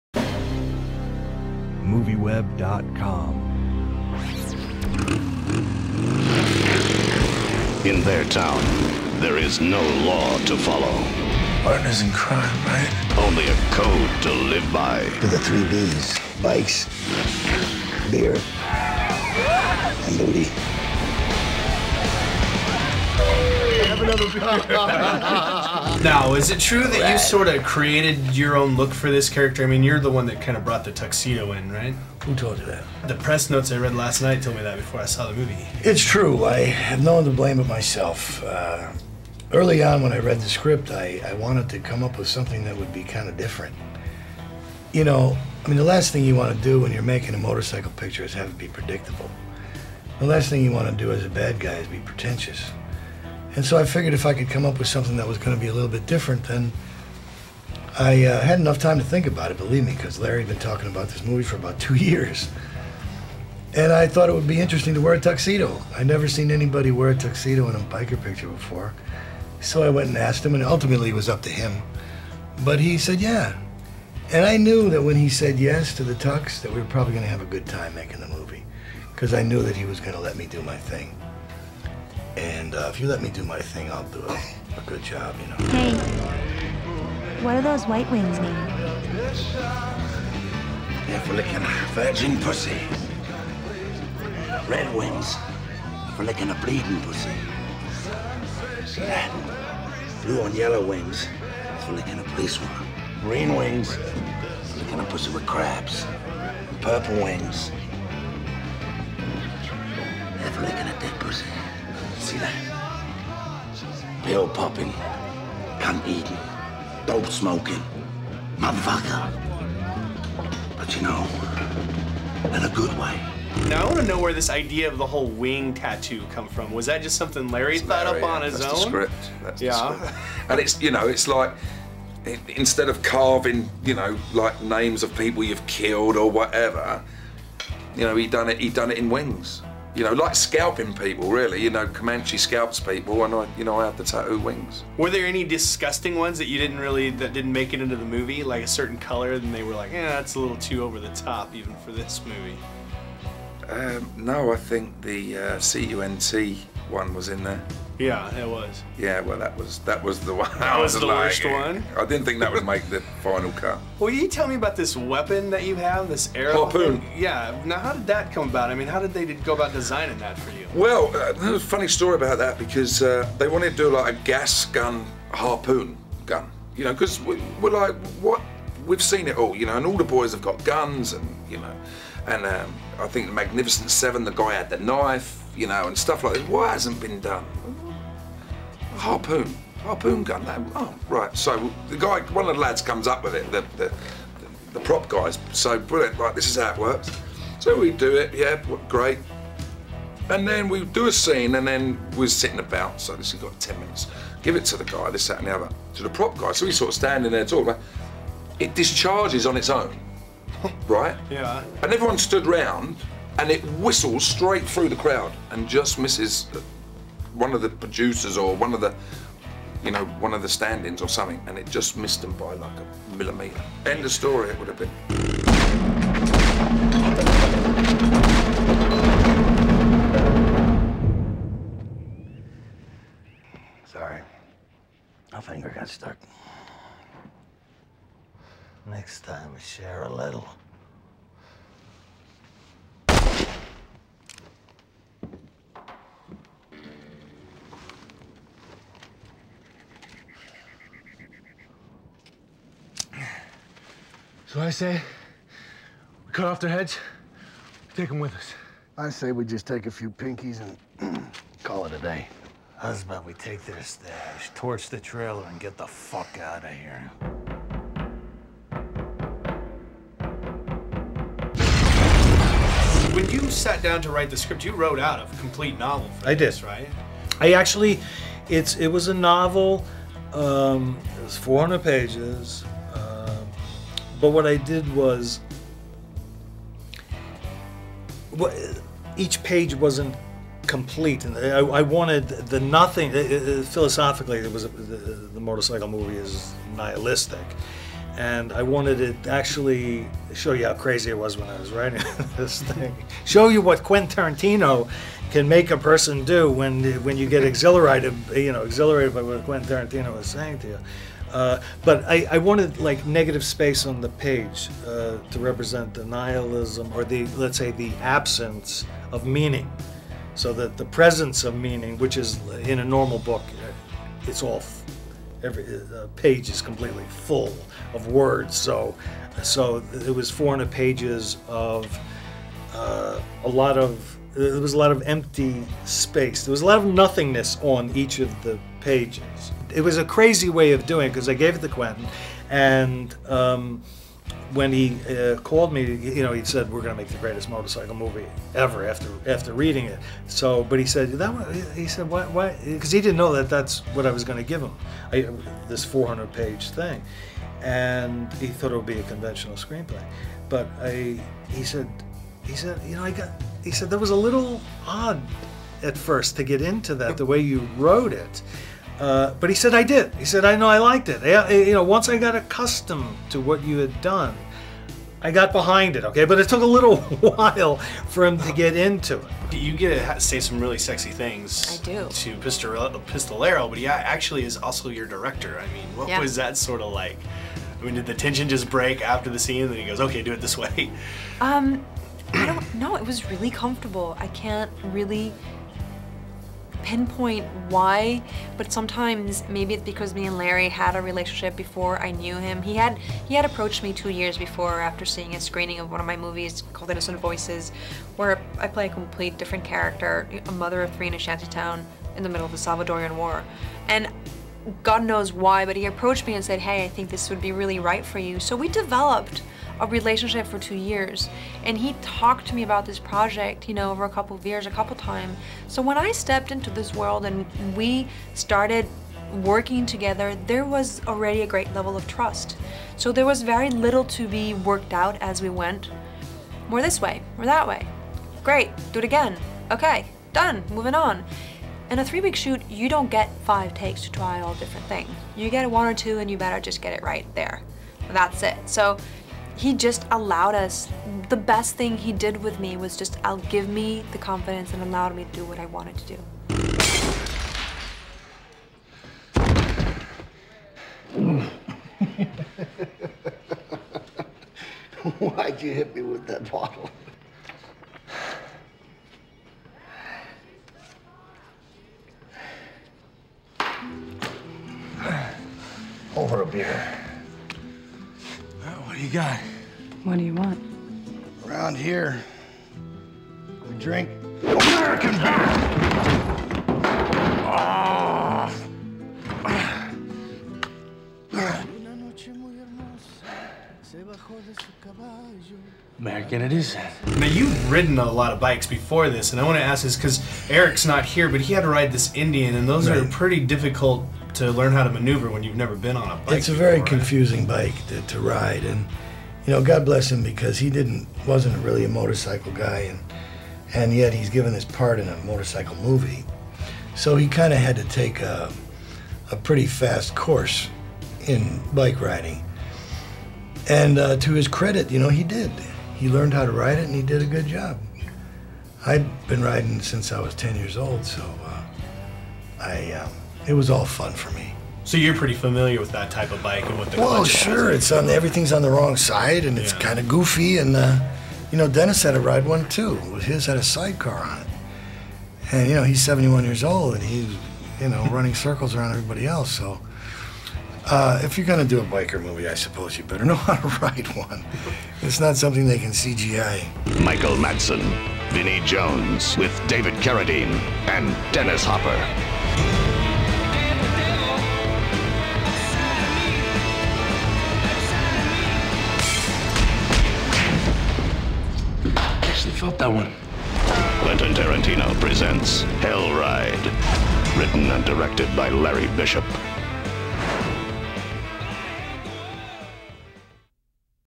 Hell Ride - Interviews with Larry Bishop and Leonor Varela and Vinnie Jones
We met up with the crew behind this hellish ride, including writer/director/actor Larry Bishop, Michael Madsen, Leonor Varela, Vinnie Jones.